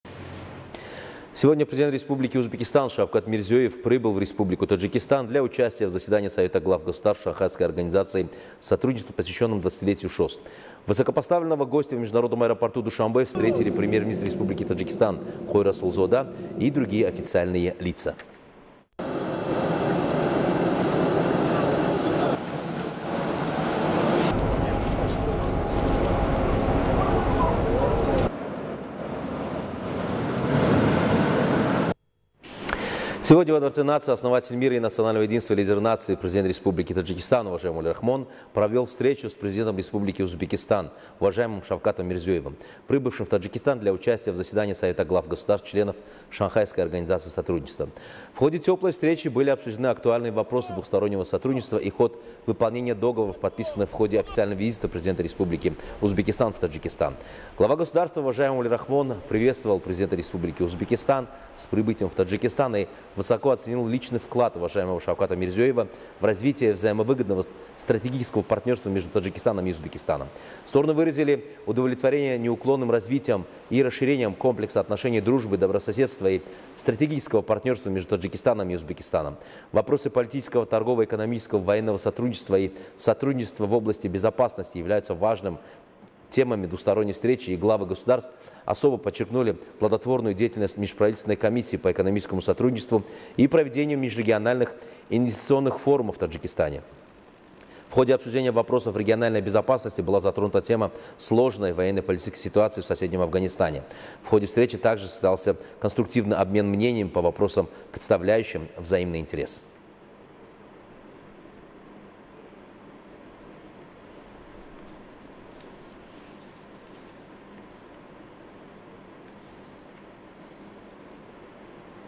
Президент Узбекистана Шавкат Мирзиёев прибыл в Душанбе / новости таджикистана
16 сентября Президент Республики Узбекистан Шавкат Мирзиёев прибыл в Республику Таджикистан для участия в заседании Совета глав государств-членов Шанхайской организации сотрудничества, посвященном 20-летию ШОС. Об этом сообщает корреспондент НИАТ «Ховар» из Международного аэропорта Душанбе.

Высокопоставленного гостя в Международном аэропорту Душанбе встретили Премьер-министр Республики Таджикистан Кохир Расулзода и другие официальные лица.

Напомним, что 17 сентября в столице Таджикистана – городе Душанбе состоится заседание Совета глав государств-членов ШОС, посвященное 20-летию образования этой организации.